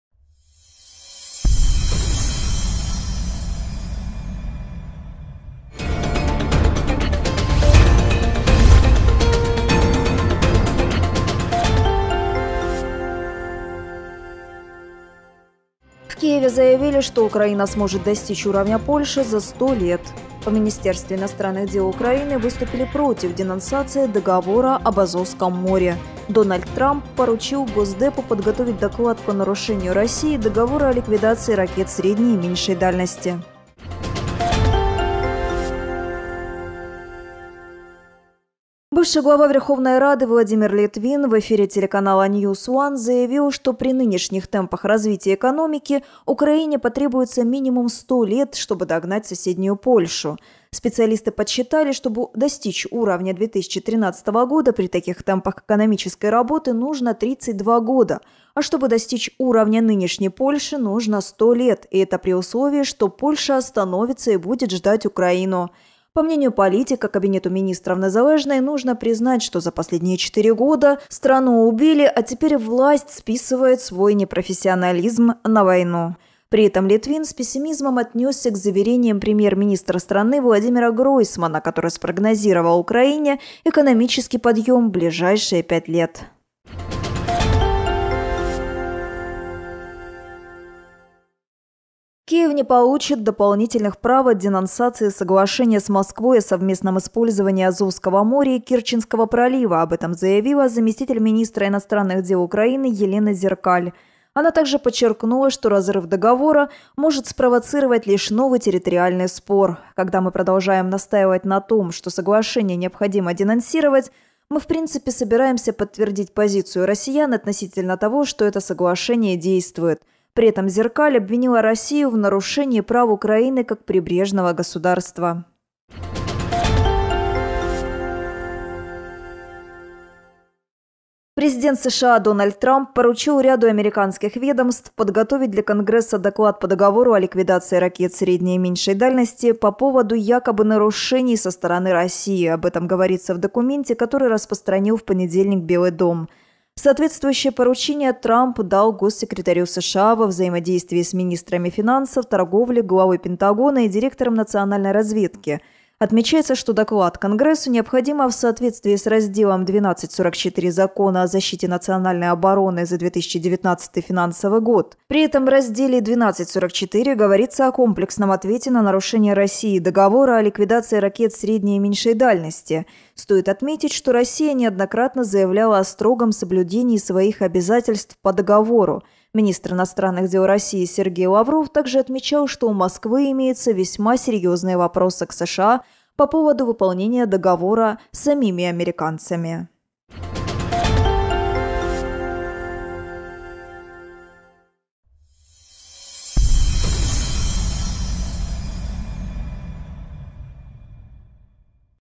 Лента Новостей 30 октября 2018 года
1) В Киеве заявили, что Украина сможет достичь уровня Польши за 100 лет
2) В МИД Украины выступили против денонсации договора об Азовском море
3) Трамп поручил госдепу подготовить доклад по "нарушению" Россией Договора о ликвидации ракет средней и меньшей дальности